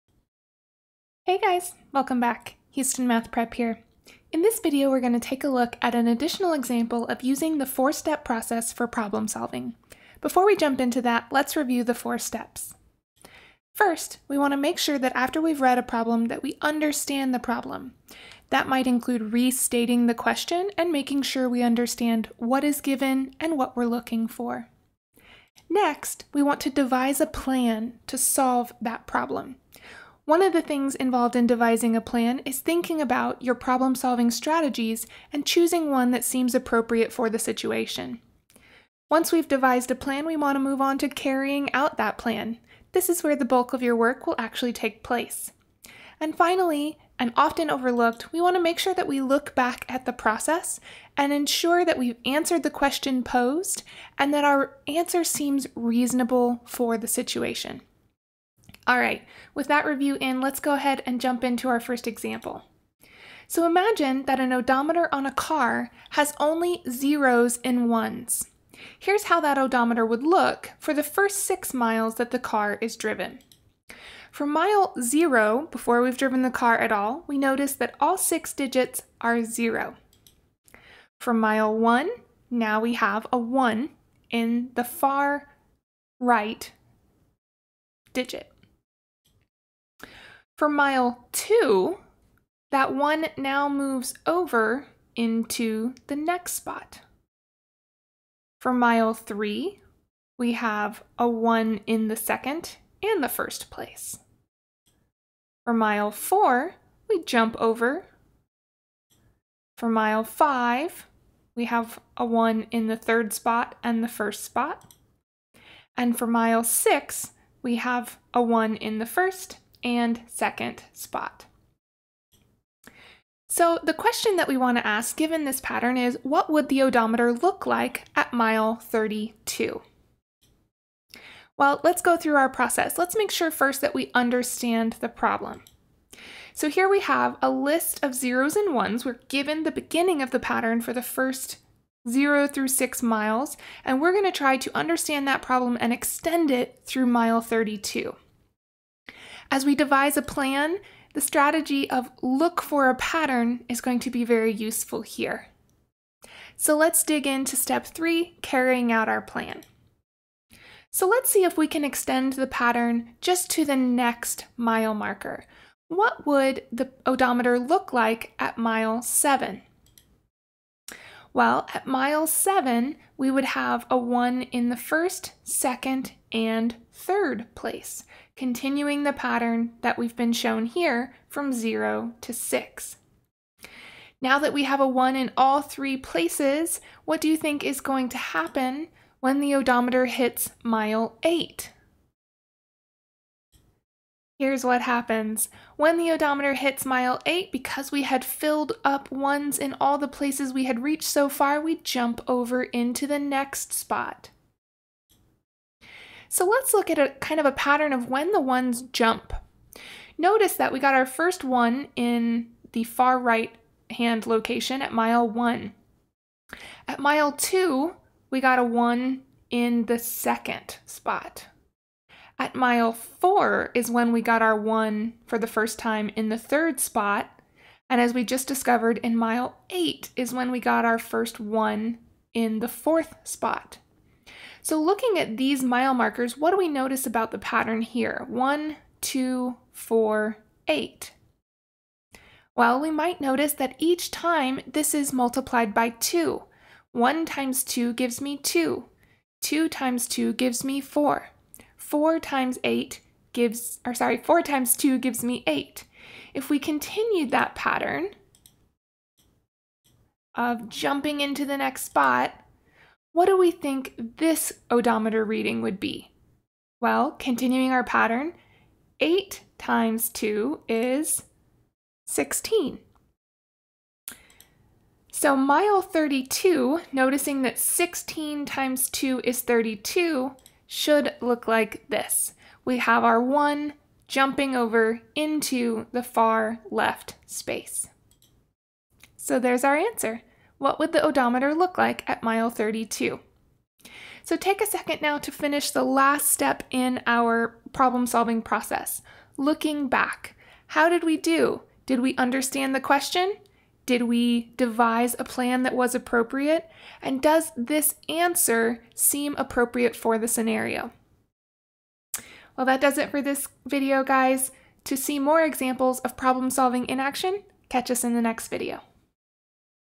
Problem Solving (Look for a Pattern)
This foundations of math video explains the four-step process of problem solving:  understanding the problem, devising a plan, carrying out the plan, and looking back.
1)  Understanding the problem involves possibly restating the problem, identifying what is given, and deciding what we are solving in the problem
2)  Devising a plan to solve the problem includes thinking about the problem solving strategies that might be appropriate for finding a solution
3)  Carrying out the plan is where most of the work takes place in completing tasks to work through the devised plan
4)  Looking back will allow us to make sure that we have solved the given problem and reflect on whether or not the solution we found is reasonable and appropriate